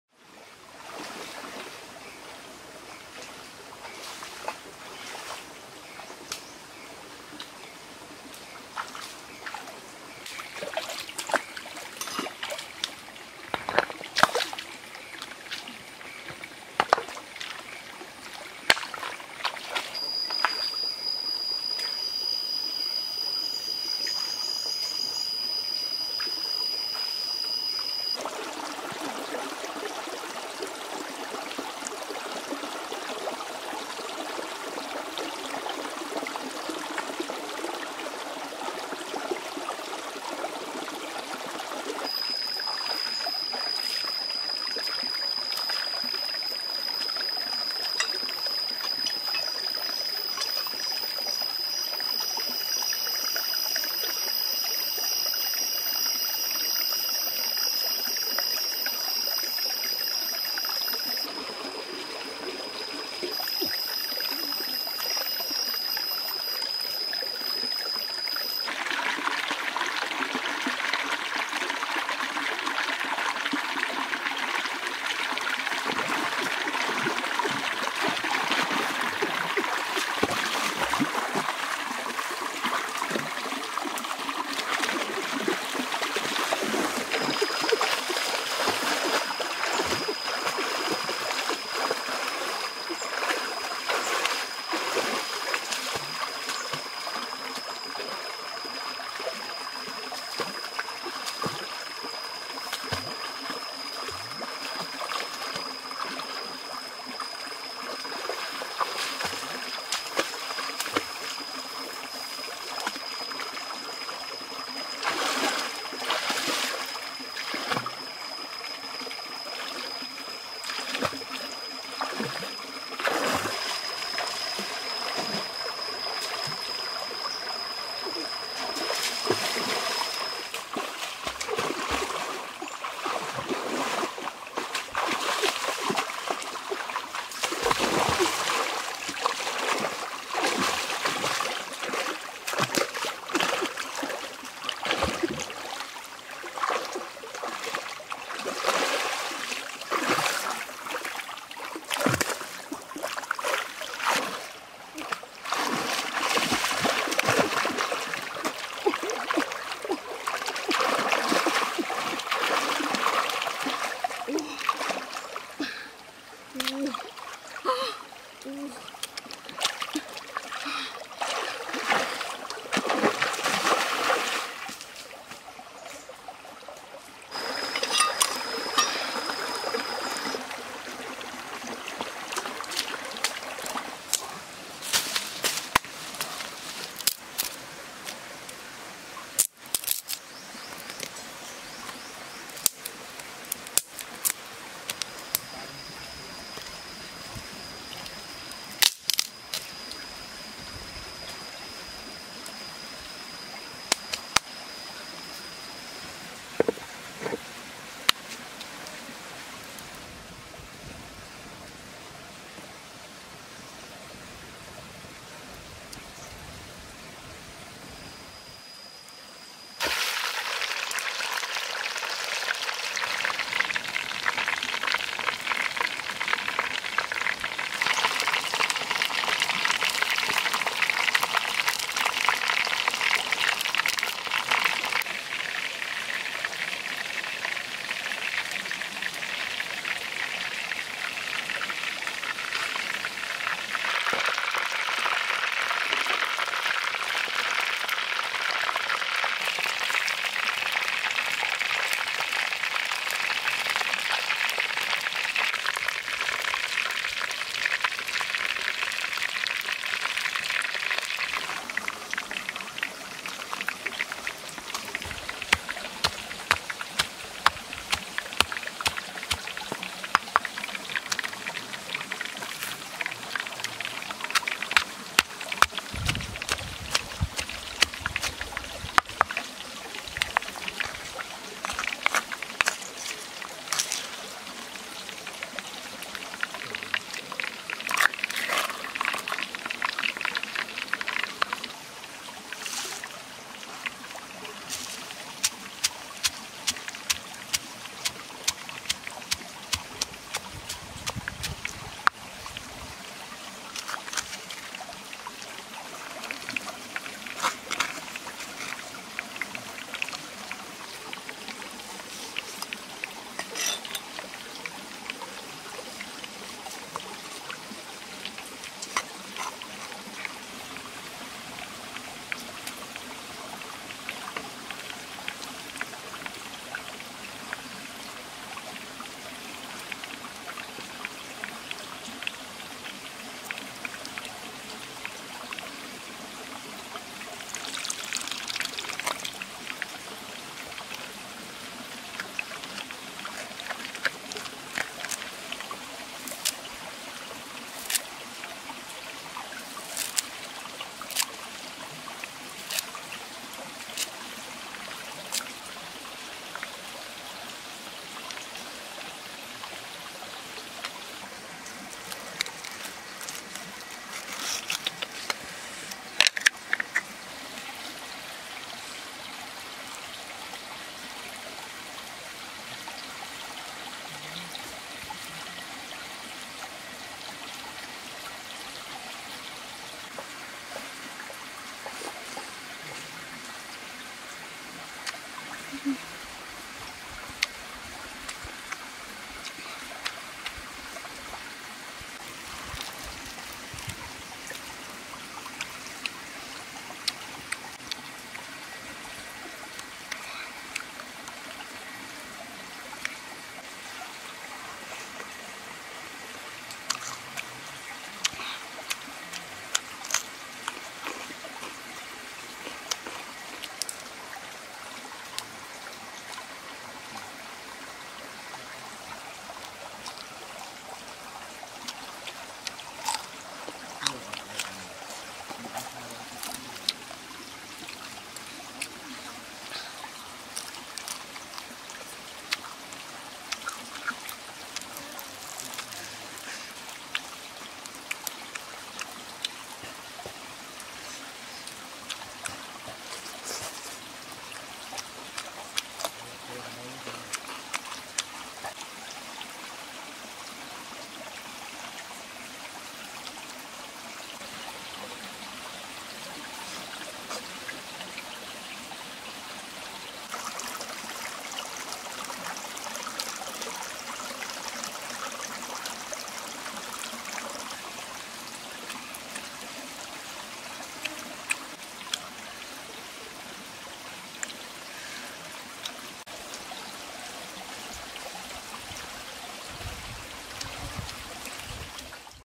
Catch big fish for food of survival in the forest - Cooking big fish with salads for lunch
Hi to day I want to show you about videos: Cooking big fish with salads for lunch